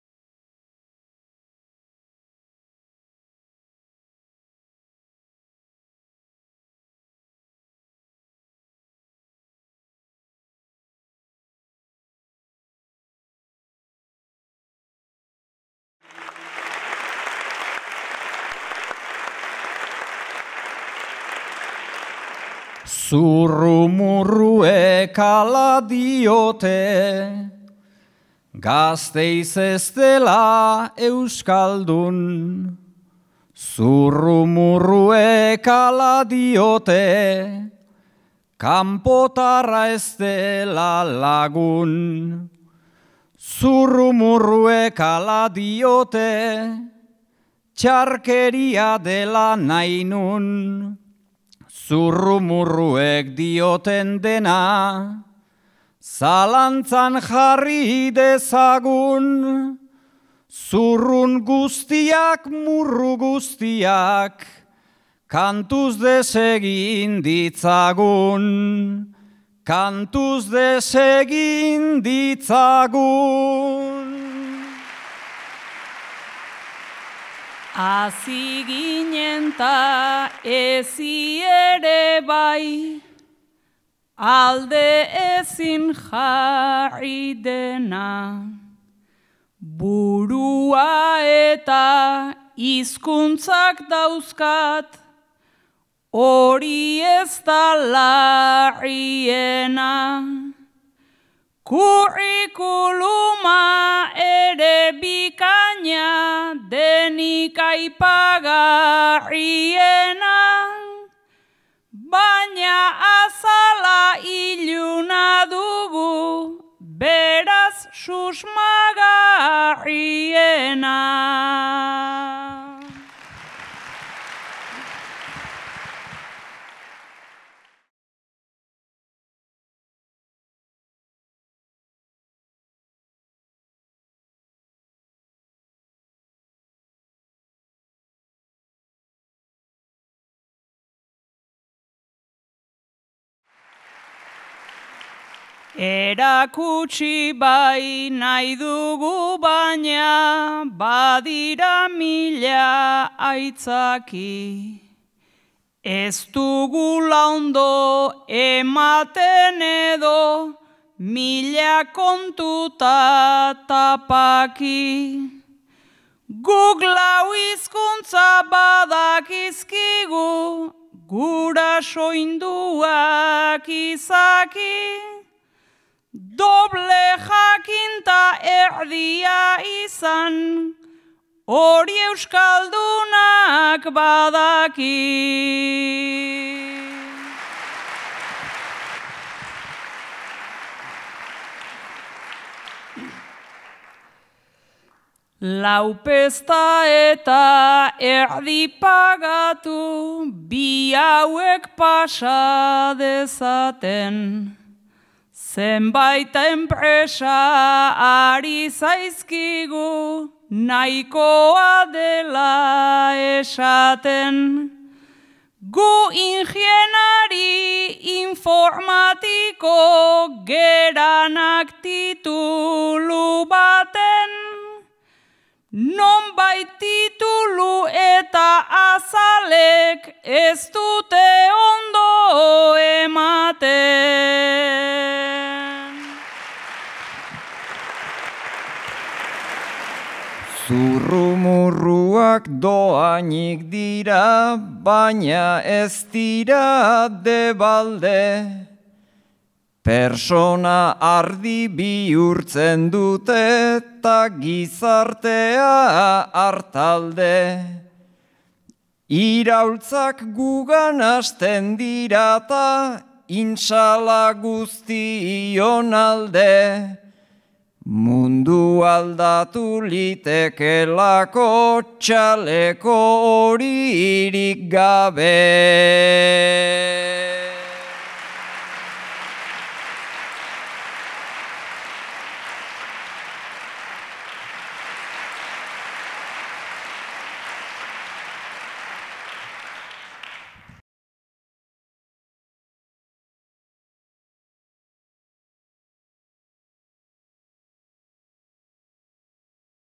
Zurrumurruen aurkako bertso-saioa. 2018-12-18 - Selección de fragmentos
2018ko abenduaren 18an Gasteizen egindako zurrumurruen aurkako
Bertso-saioaren unerik esanguratsuenak bildu dira bideo honetan.
Estereotipoei aurre egiteko helburuarekin Aitor Mendiluze, Aitor
Sarriegi, Amets Arzallus, Beñat Gastelumendi, Igor Elortza, Mailaen
Lujanbio, Sustrai Conina eta Una Agirrek parte hartu zuten eta 500
pertsona baino gehiago bertaratu ziren.

El video recoge los momentos más significativos del Bertso-saioa
Antirumores celebrado en Vitoria-Gasteiz el 18 de diciembre de 2018 con
la participación de Aitor Mendiluze, Aitor Sarriegi, Amets Arzallus,
Beñat Gastelumendi, Igor Elortza, Mailaen Lujanbio, Sustrai Colina y
Unai Agirre, con la finalidad de combatir estereotipos y con la
asistencia de más de 500 personas.